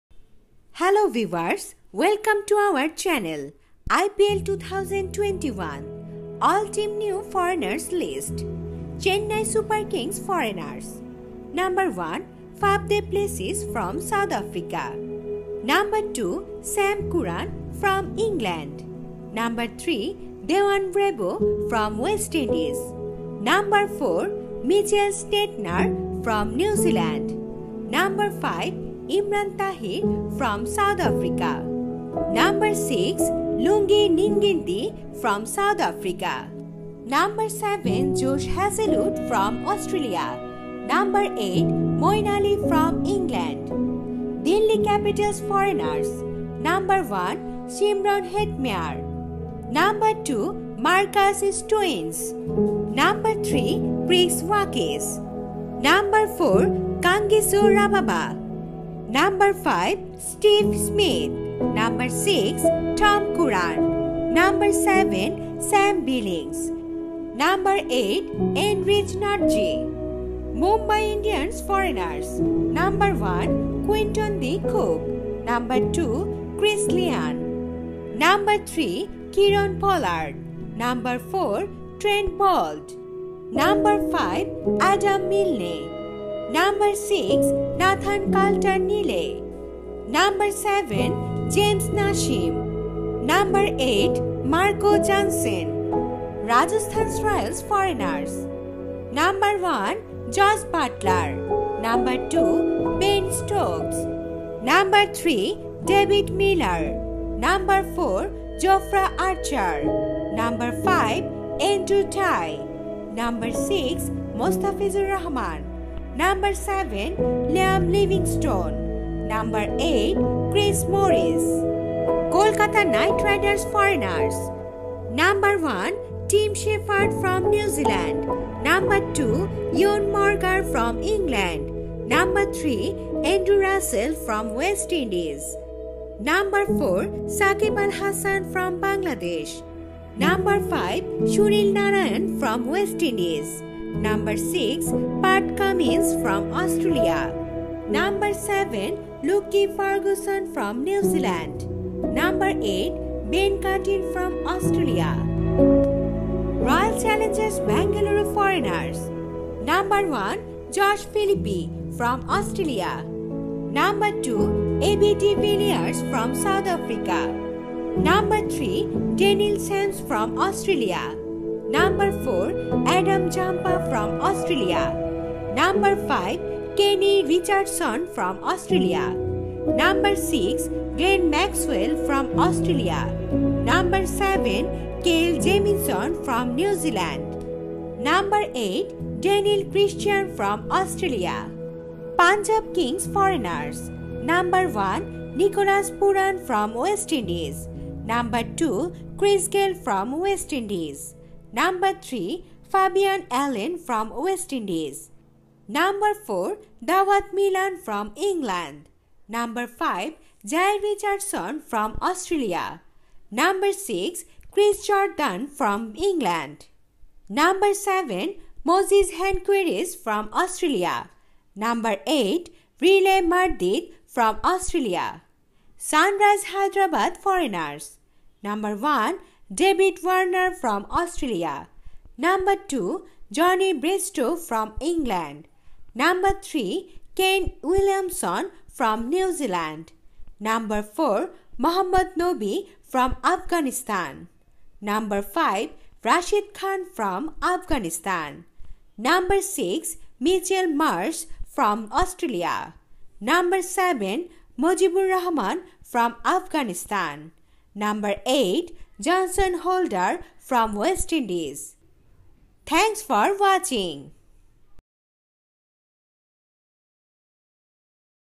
IPL 2021 All Team New Foreign Player List | All Team Squad For IPL 2021 | Expensive Foreign Player
IPL 2021 All Team New Foreigner List | All Team Squad For IPL 2021 | KKR, CSK, MI, DC, RR,